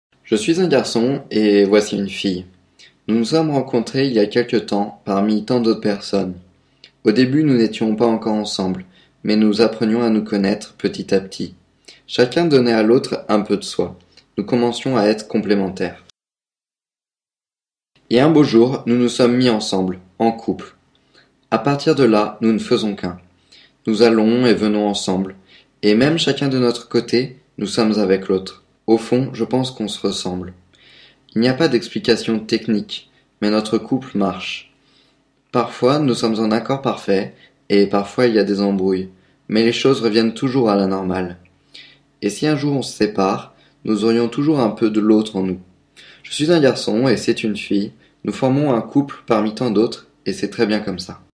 La démo du couple
C'est la démo du couple. Pas le couple modèle, mais un modèle de couple. Une démo en patte à modeler.